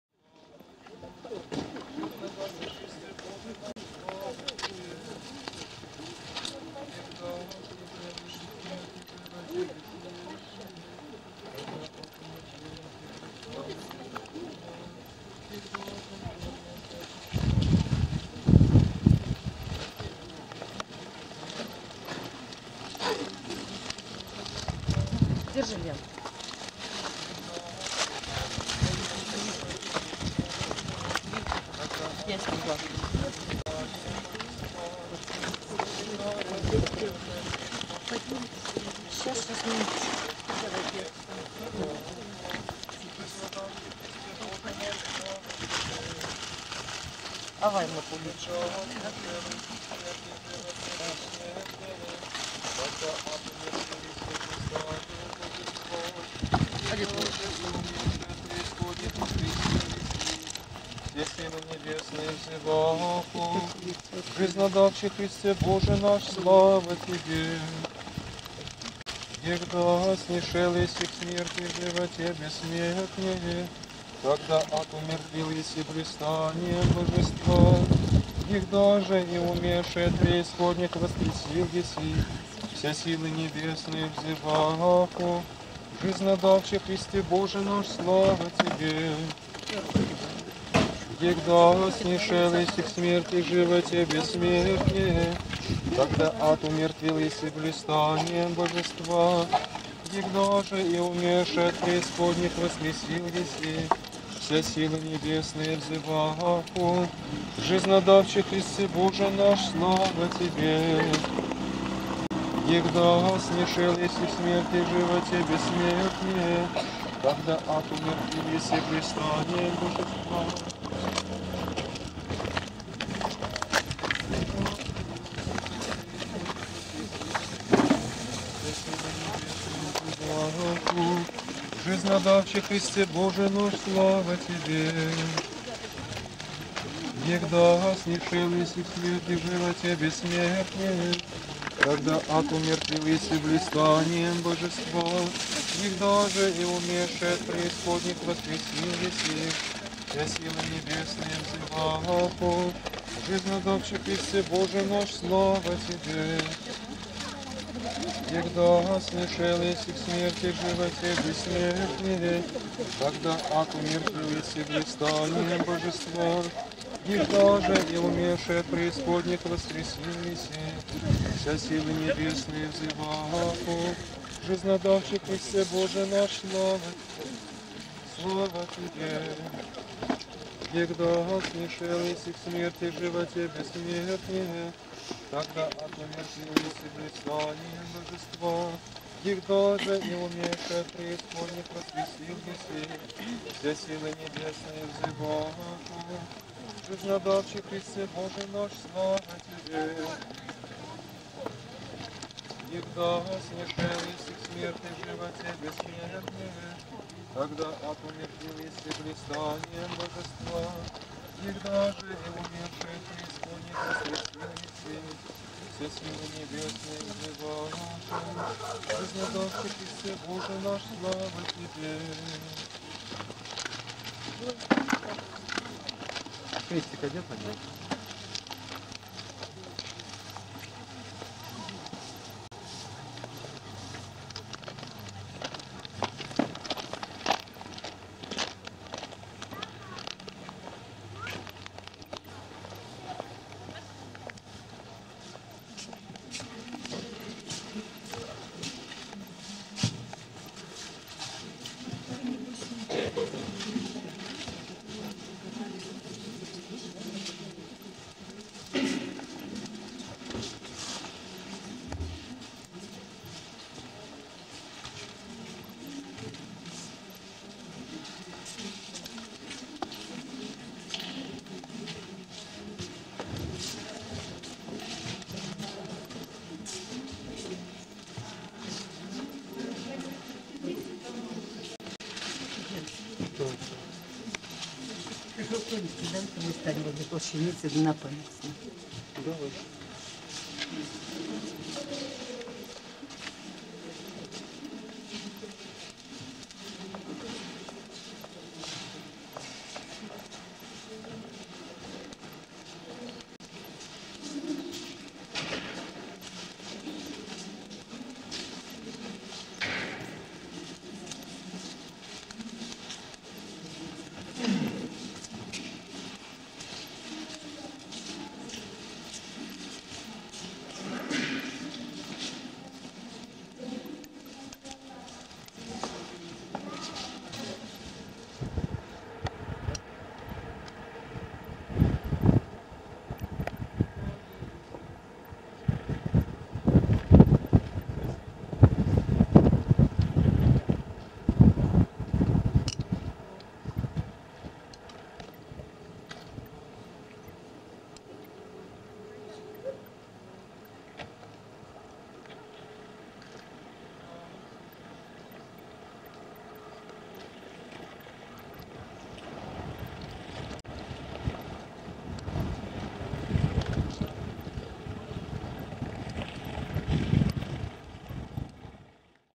Великая Суббота Страстной Седмицы 26 апреля 2003
освящение куличей, пасох, Плащаница